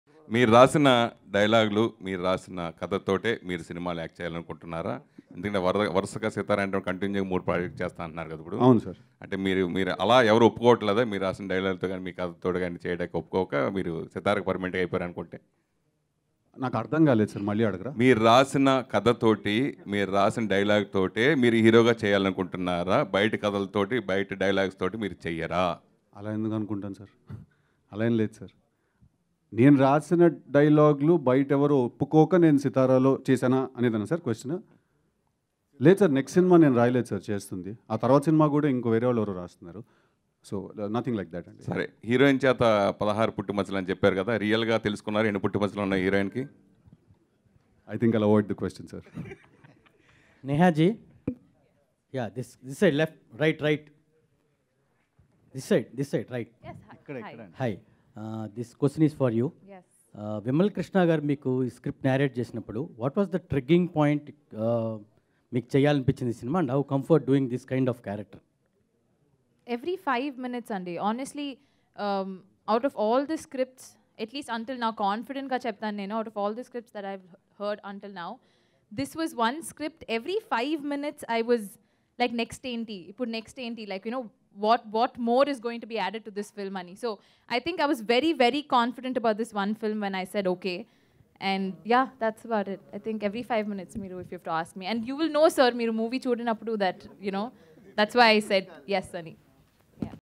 Journalist Suresh Kondeti Asking Controversial Question Hero Siddhu | DJ Tillu | Cinema Culture
Watch Here Journalist Suresh Kondeti Asking Controversial Question Hero Siddhu